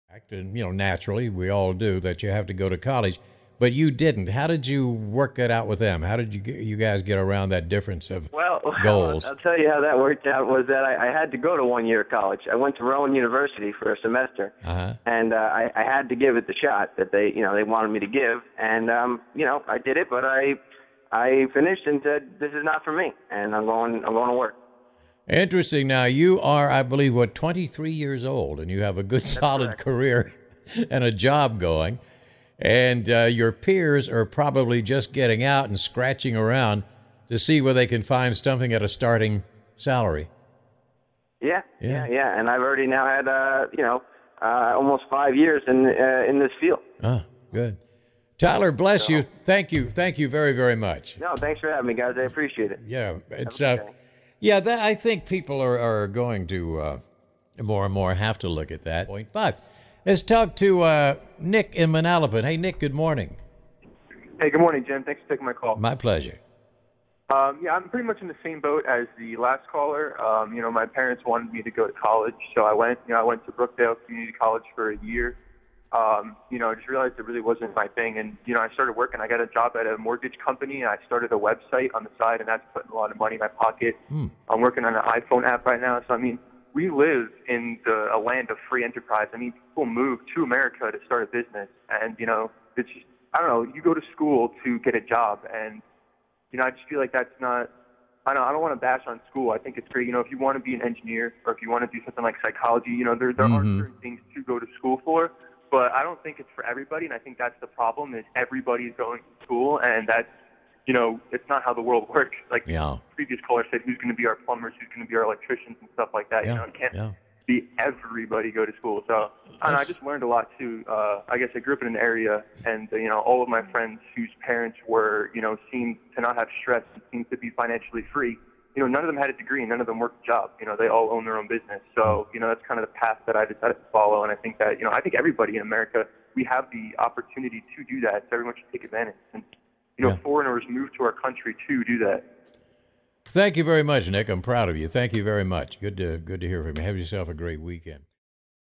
Millennials share inspiring success stories about ditching college
New Jersey 101.5 callers share their inspiring stories about choosing not to attend college. (Photo: Chris McGrath/Getty Images News)